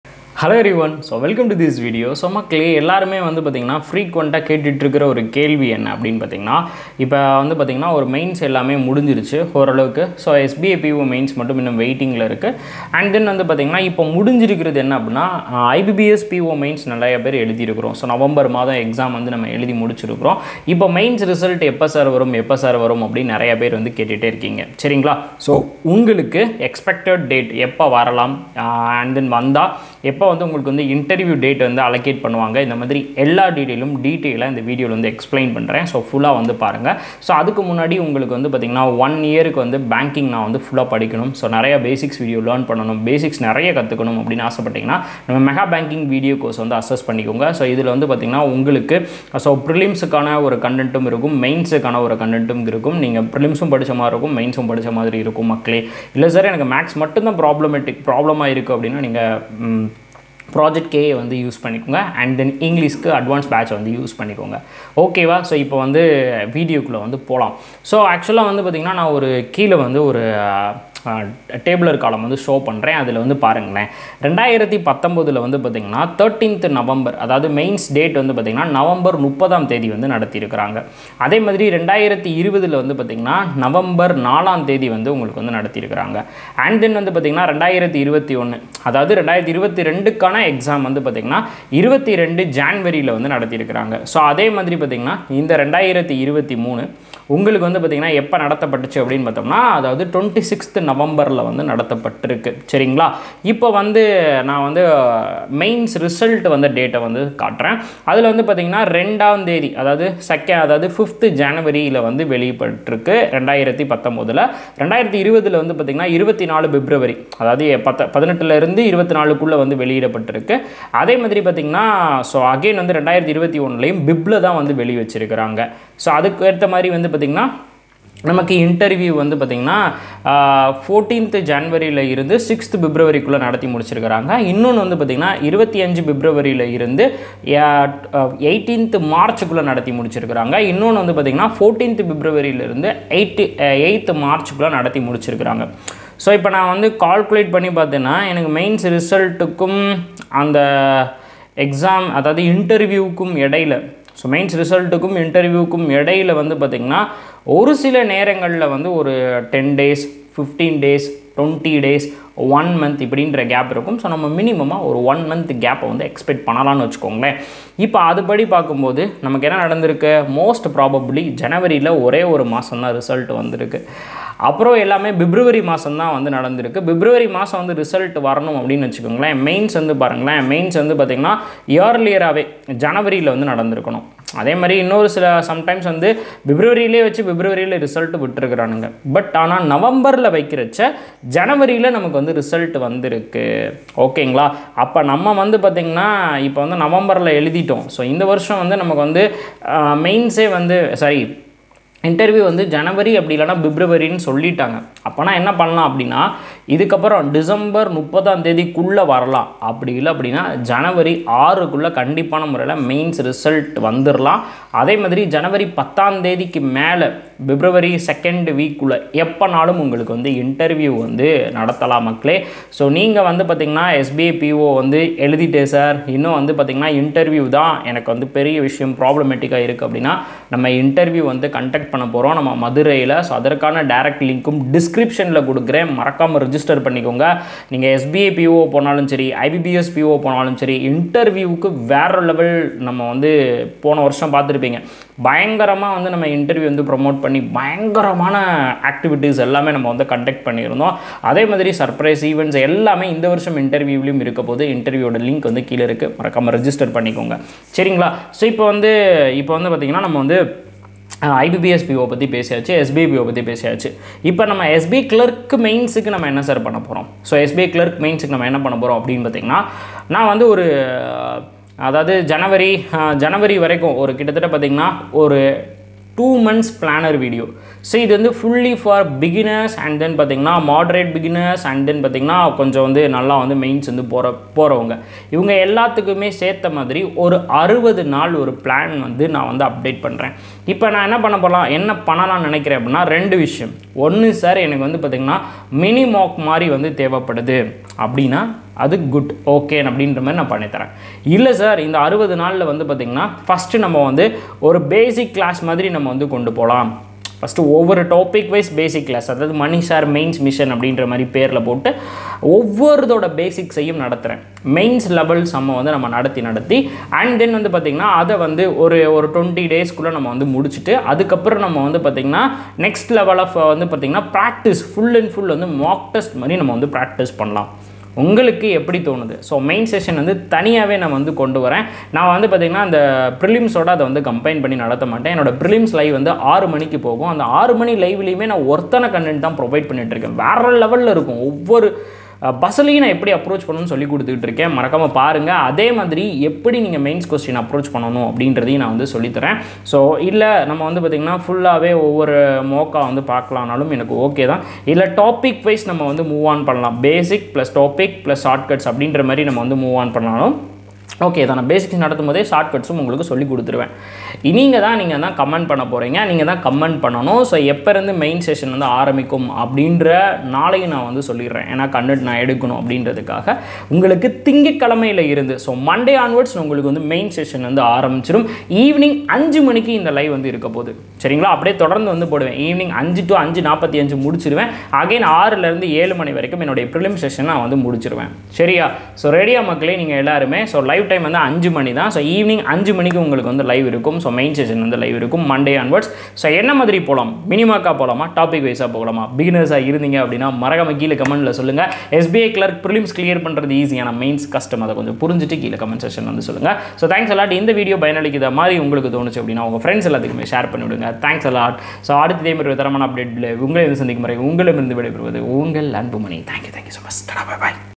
Expected IBPS PO Mains Result Date? 💯| Important Update About SBI Clerk Mains!📢 | Mr.Mani
🌐⏩Direct Link to See KR LOGICS Online academy/ mentorship programme Plans limited seats kindly enroll soon